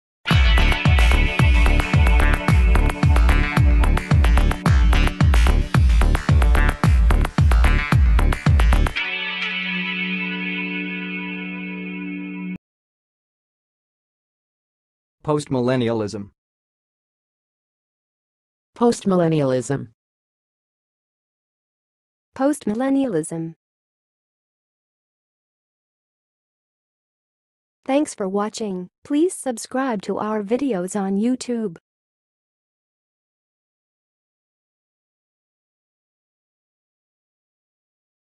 POSTMILLENNIALISM - HOW TO PRONOUNCE POSTMILLENNIALISM?
POSTMILLENNIALISM - Avoid mistakes: The right way to say Postmillennialism in 2023

Postmillennialism is a theological belief within Christian eschatology that asserts that the second coming of Jesus Christ will occur after a period of spiritual and cultural transformation, during which the world will gradually become more Christianized.
According to postmillennialists, this transformation will be facilitated by the spread of the gospel and the establishment of God's righteousness on earth.
Postmillennialism stands in contrast to other views like premillennialism and amillennialism, which hold different perspectives on the timing and nature of Christ's return.

Pronunciation:
To pronounce postmillennialism, you can say 'post-mil-len-ee-al-ism'.
Other alternative pronunciations include 'post-mill-ee-al-ism' or 'post-mi-len-nee-al-ism'.

Test yourself by speaking the following examples:
1. Postmillennialism emphasizes the belief that the world will progressively improve through the spread of the gospel and the influence of Christianity
2. Many Reformed theologians, such as John Calvin and Jonathan Edwards, held postmillennial views
3. A postmillennialist interpretation of Revelation sees it as a depiction of the ongoing spiritual victory of Christianity and the eventual triumph of Christ's kingdom on earth